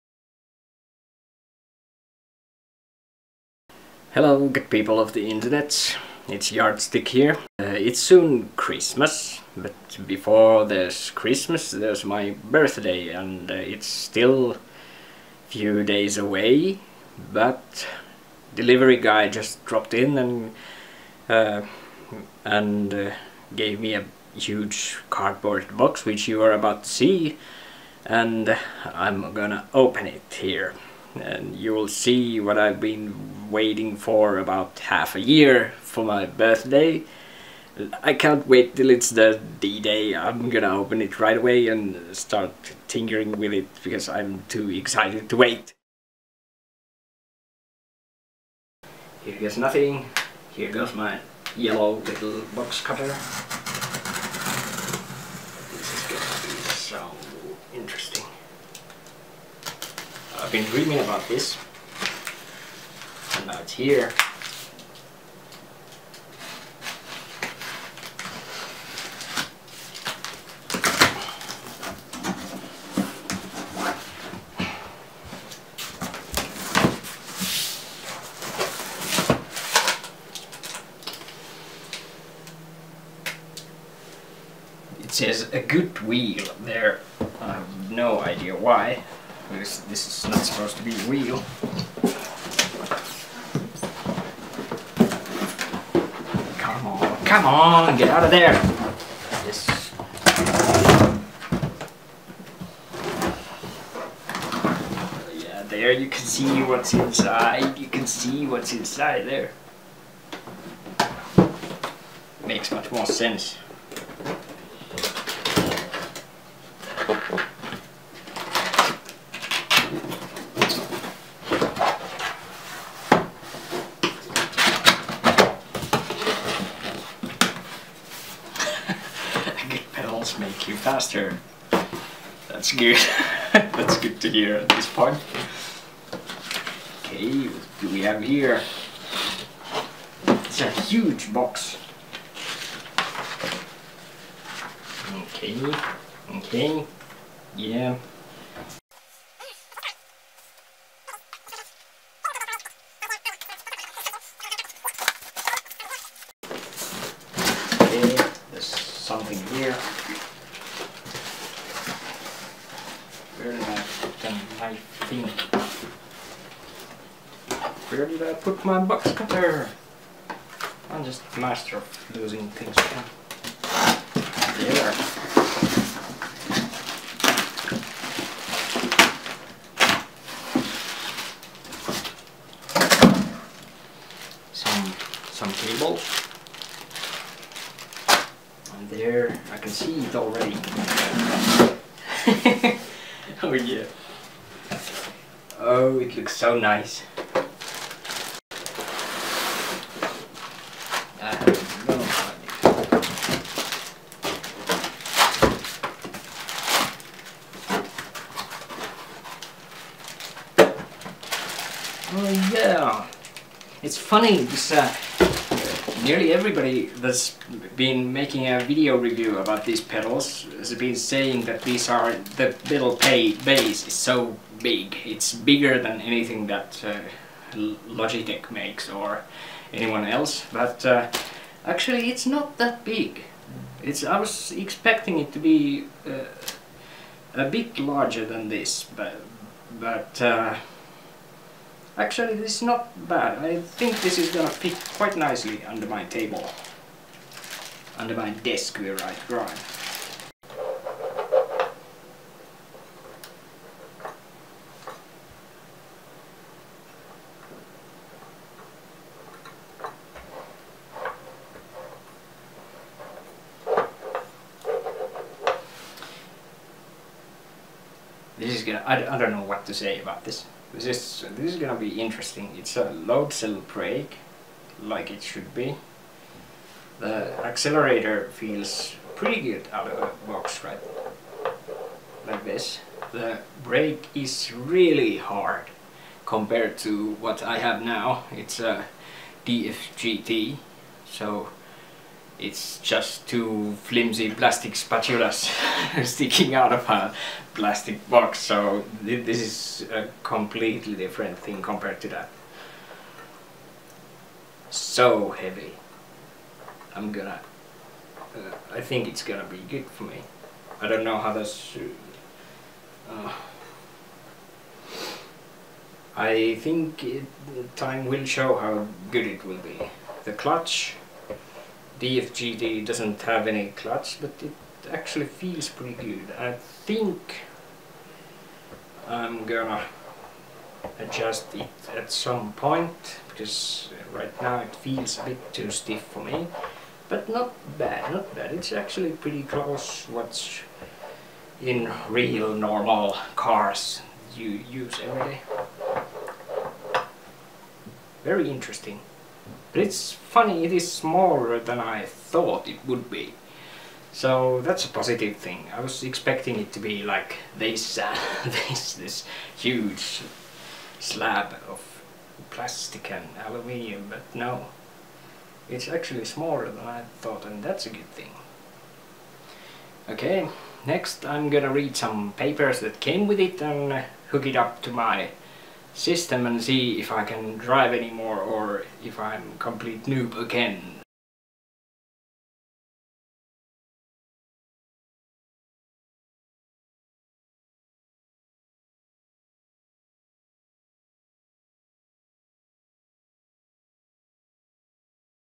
Fanatec Unboxing
Unboxing Fanatec CSR Elite pedals.
Birthday present for my self.